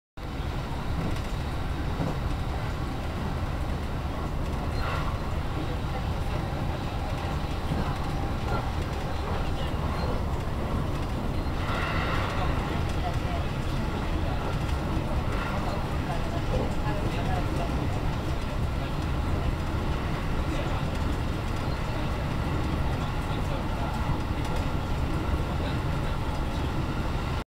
221系非改善車 車内LED 広告・啓発表示～ホーム上事故防止～
221系の車内LED表示機は駅間でJR西日本からのお知らせが流れます。
【内容】
お客様へのお願い。線路への転落や列車との接触事故防止のために、ホームでは黄色い点字ブロックの内側をご通行ください。特にスマートフォンなどを操作しながらの歩行はたいへん危険です。皆さまのご協力をお願いいたします。

大和路快速　大和小泉～郡山